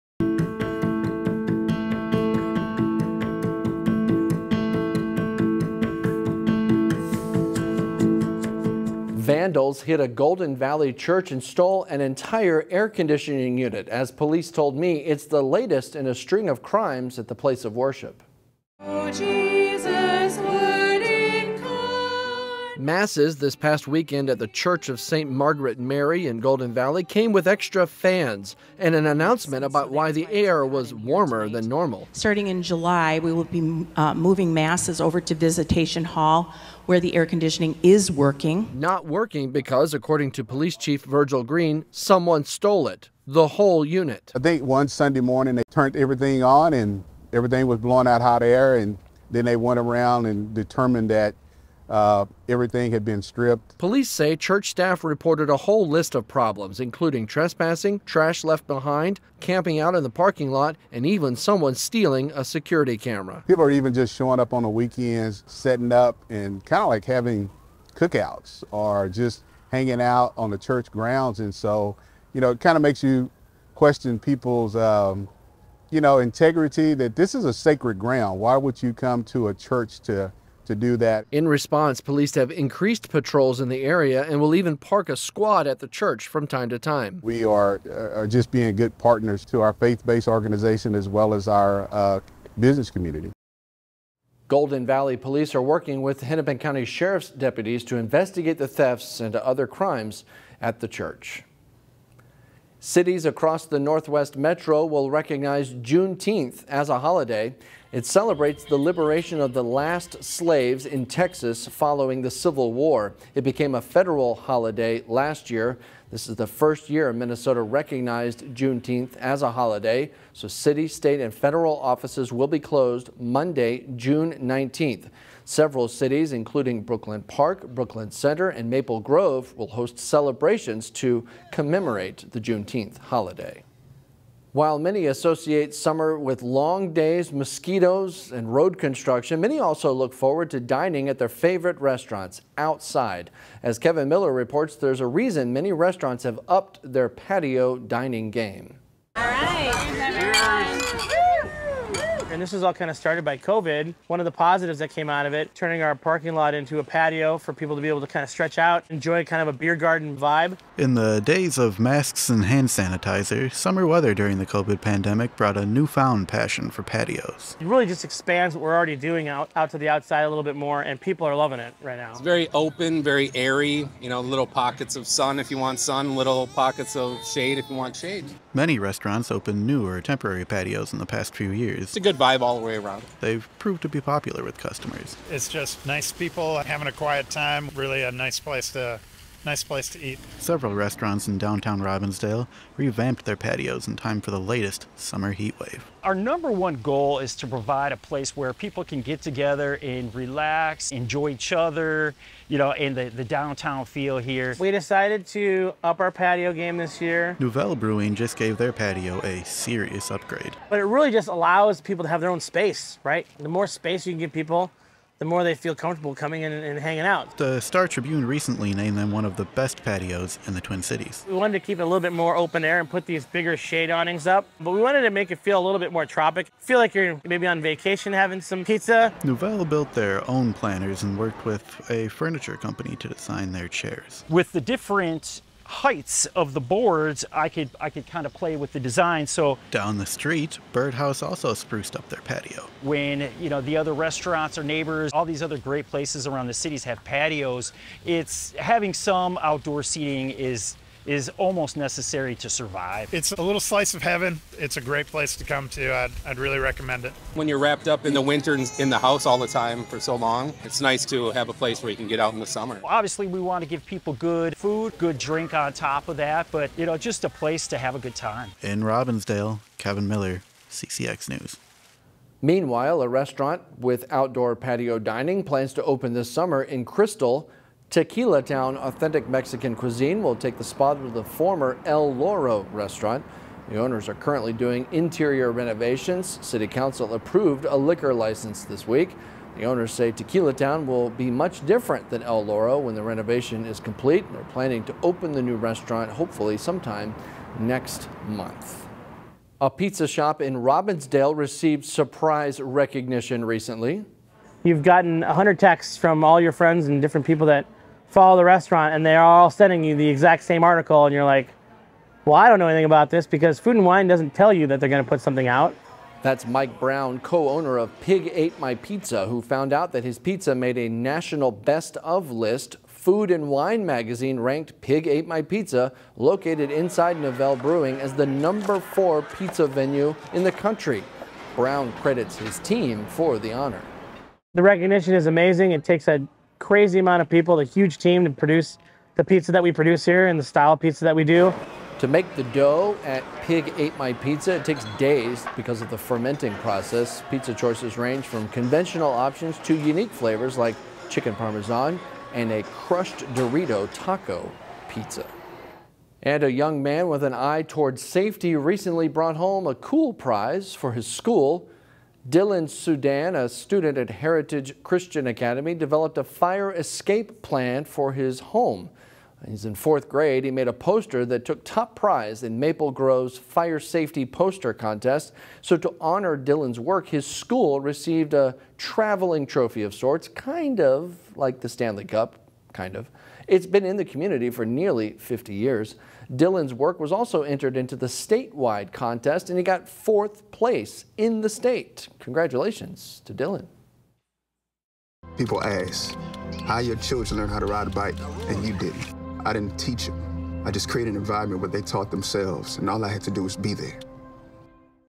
CCX News June 11, 2023
Golden Valley Church Theft Update

Local Cities Holiday Declarations for Juneteenth

Robbinsdale Restaurants Focus on Patio Dining

New Crystal Restaurant Coming Soon

Robbinsdale Pizzeria Receives National Recognition

Maple Grove Student Wins Fire Safety Contest